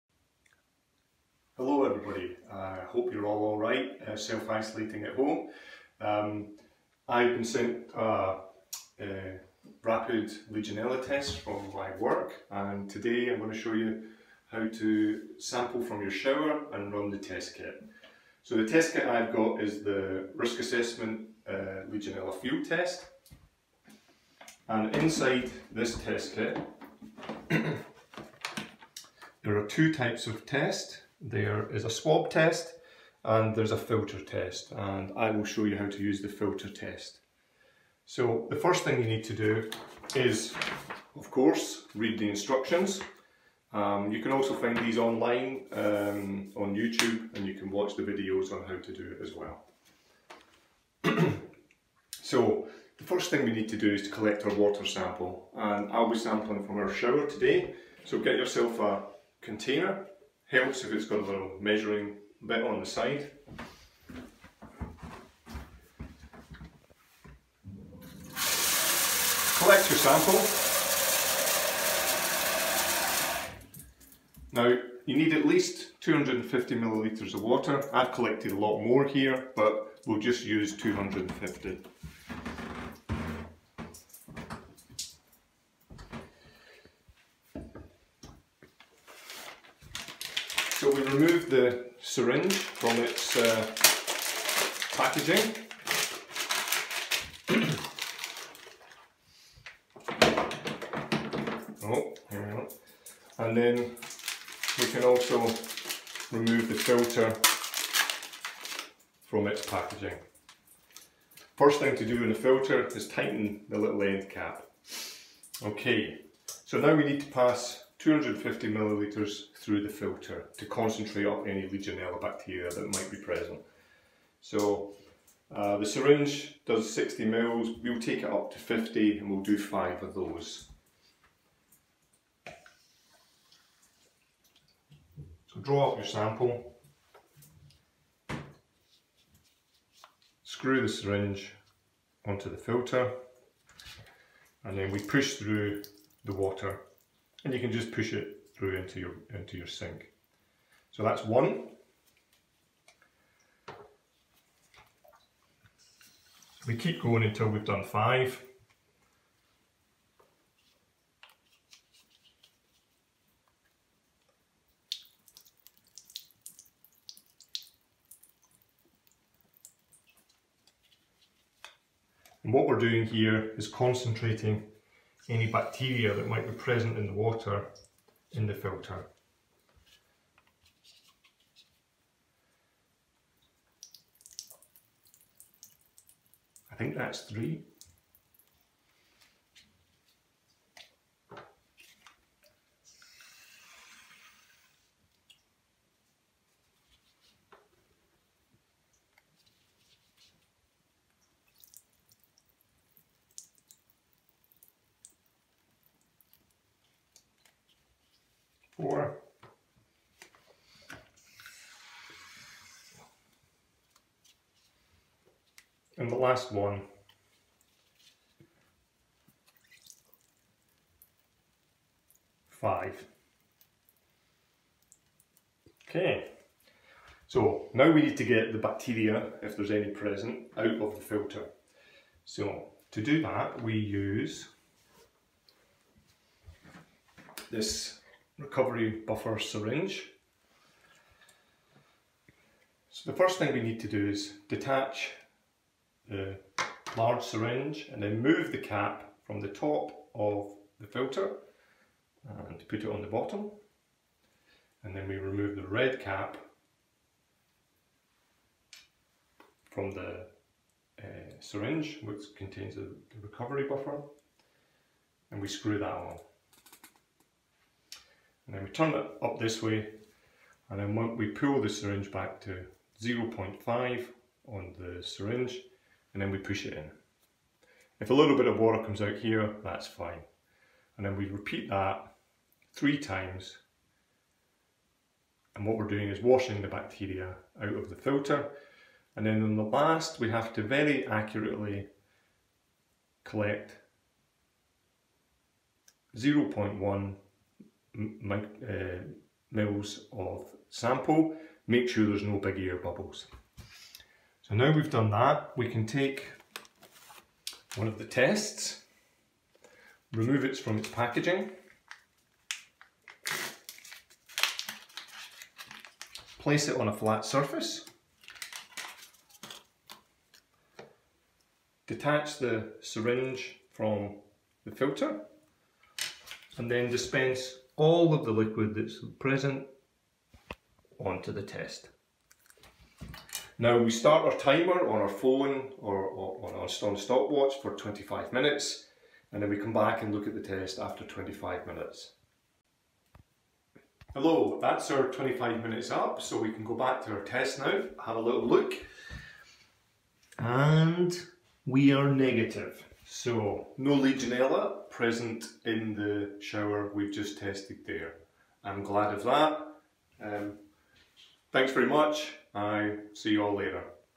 Hydrosense from home - Risk Assessment Legionella Test Kit with Richard
Hydrosense's Technical Director, Richard Campbell, demonstrates the Risk Assessment Legionella Testing Kit at home during lockdown. The test is convenient, can be used in isolation and has a high sensitivity of 100 CFU/L and 200 CFU/swabbed area. The kit can be used to maintain a high level of water safety in any water system, at home or in your business.

Hydrosense detects the most dangerous strain of Legionella bacteria, Legionella pneumophila SG1, which is responsible for practically all cases of Legionnaires' disease.